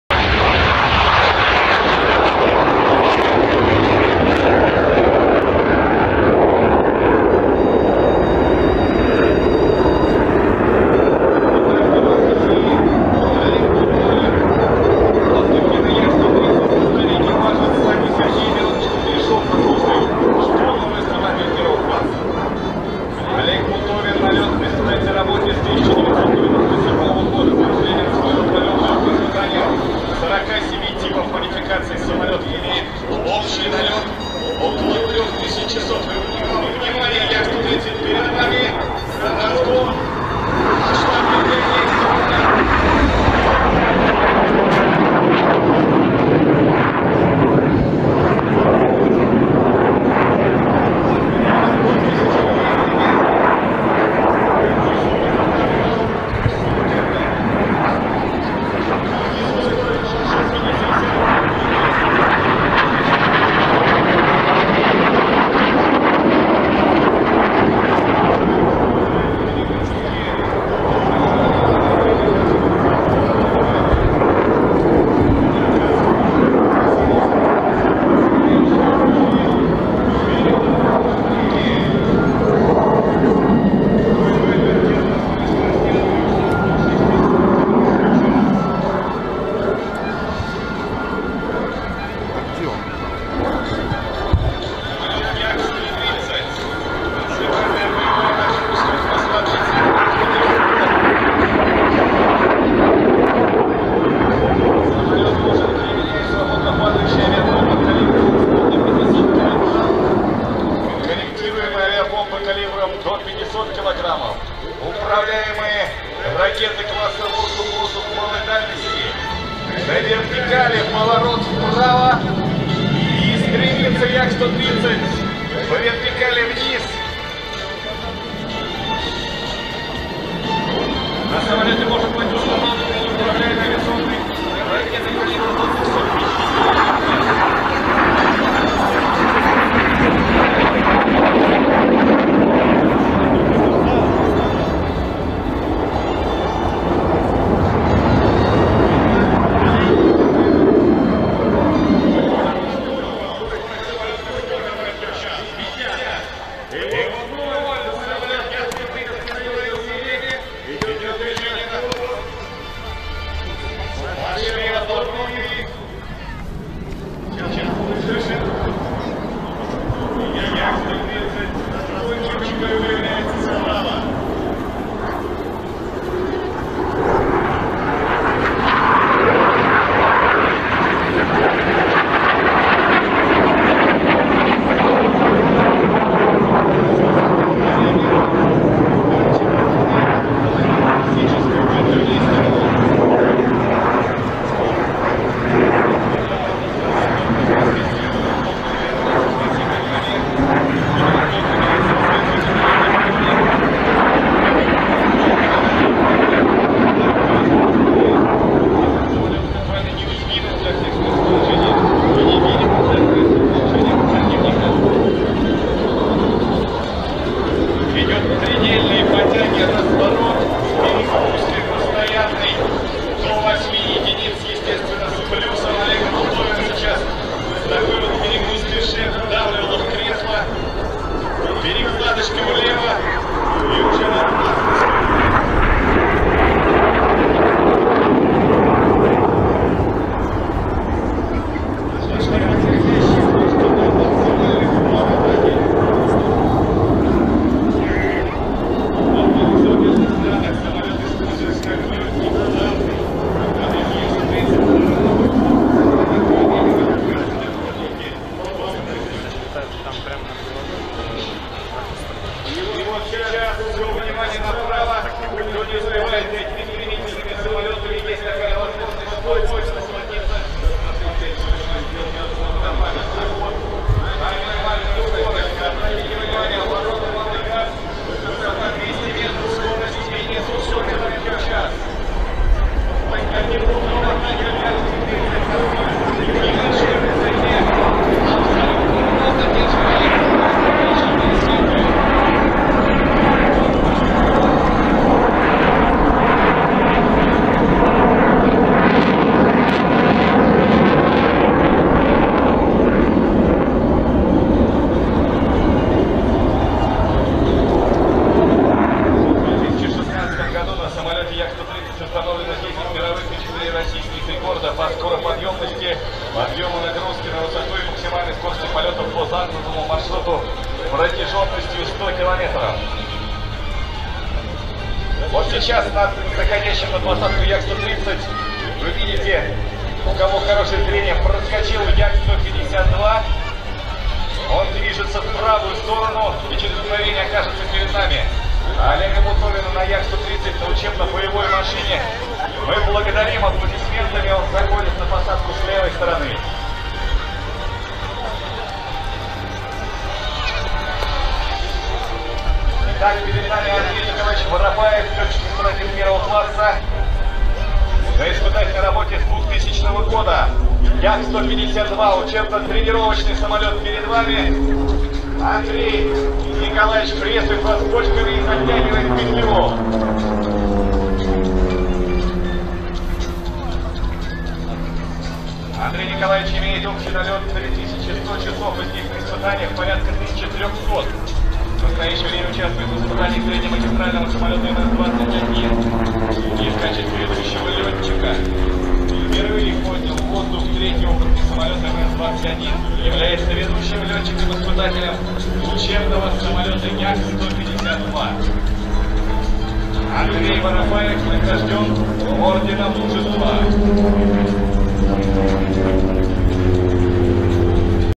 Yakovlev Yak-130 demonstration at MAKS 2019
30 August 2019
video RT